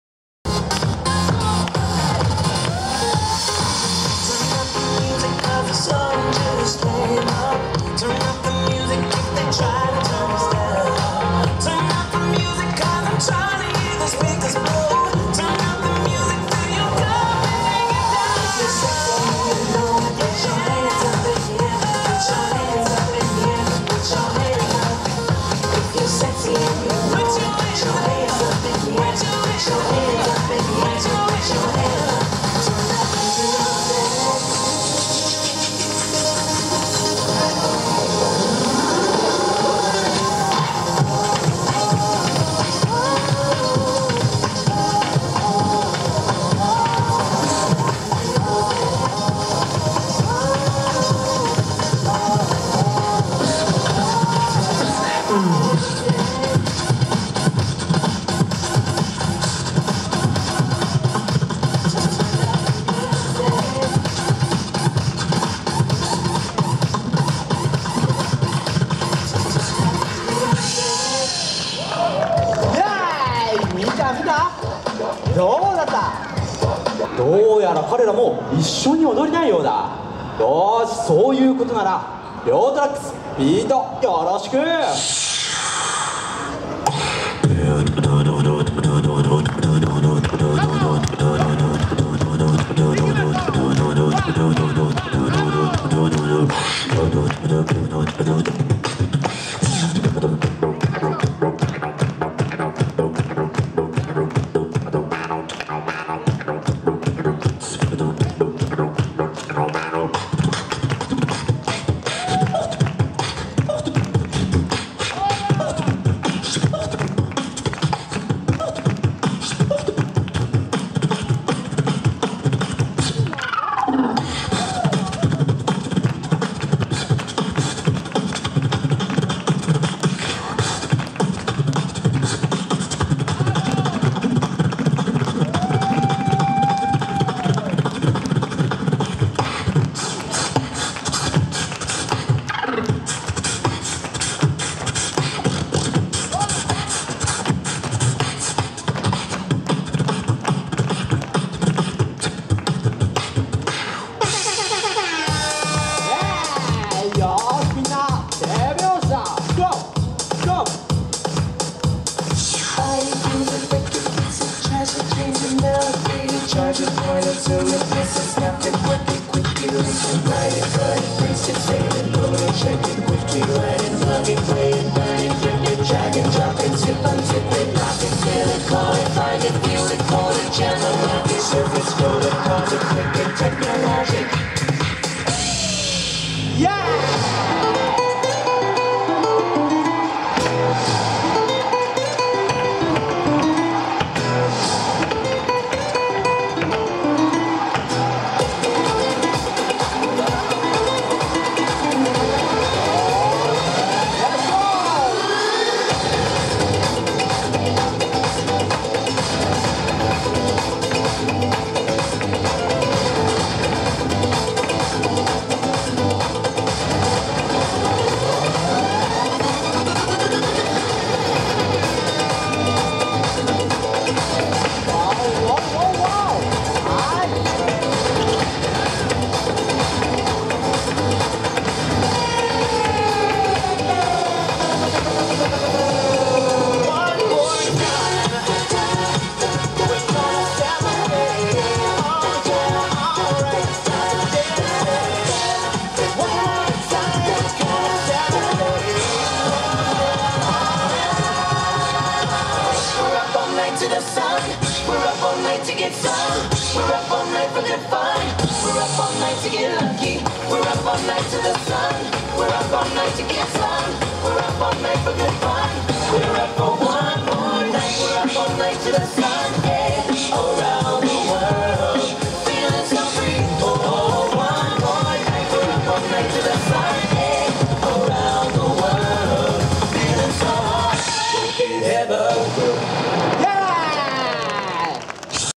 AMAZING BREAK DANCE SHOW at Universal Studios Japan
Break Dance and Soccer Show at Universal Studios Japan.

My name is Alexia and I provide video content about Japan.